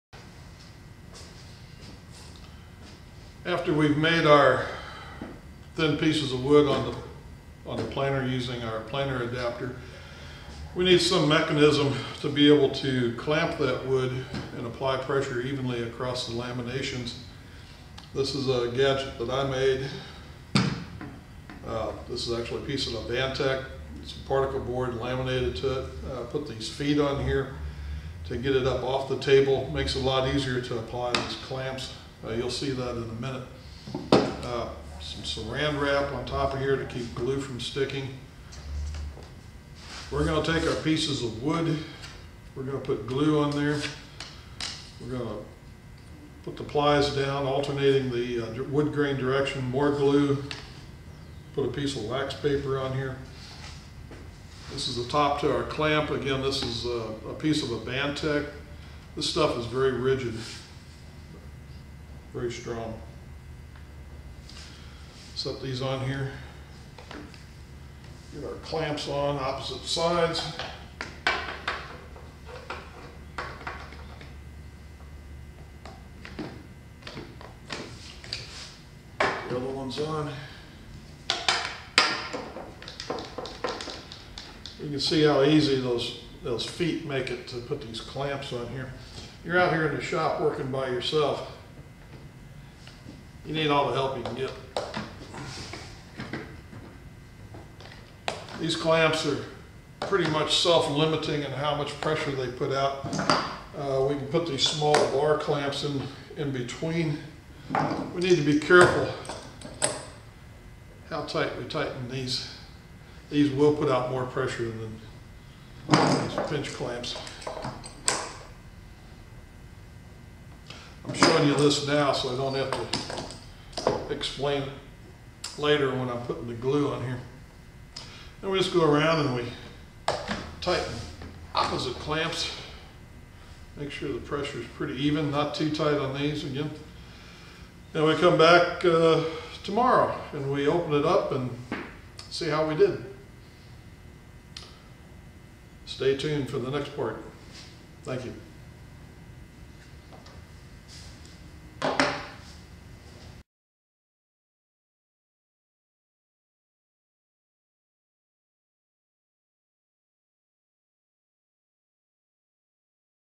Plywood Clamping Jig - Making Plywood Part 1
A process for making homemade plywood for gears in wood gear clocks.